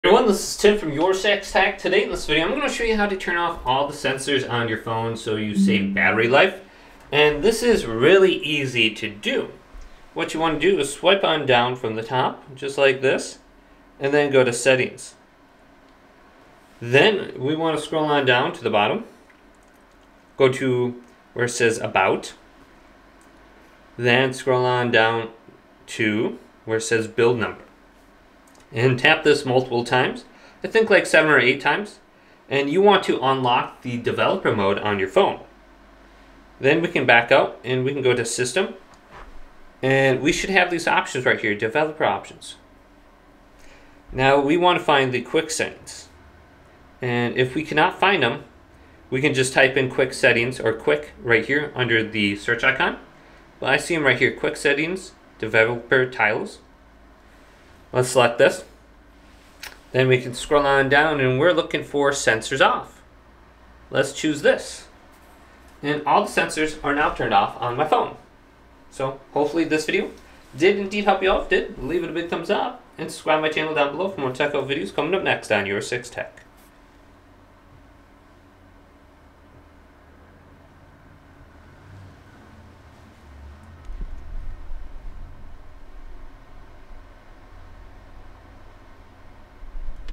How to Turn Off All Sensors on Android Phone to Save Battery (Settings Tutorial)
To disable all sensors on your Android phone and potentially save battery life, follow these steps:

Enable Developer Options:
Go to your phone’s Settings.
Scroll down and tap on About phone.
Find the Build number entry and tap it seven times to enable Developer Options.

Activate Sensors Off:

Return to the main Settings menu.
Scroll down to Advanced and select Developer options.
Look for Quick Settings Developer Tiles and tap it.
Enable the Sensors Off toggle.
Use the Sensors Off Toggle:

Pull down the notification shade from the top of your screen.
You’ll now see a new tile called “Sensors Off” in the quick settings panel.
When you enable this toggle, most sensors—including the camera, microphone, accelerometer, and gyroscope—will be turned off, providing a more private mobile experience.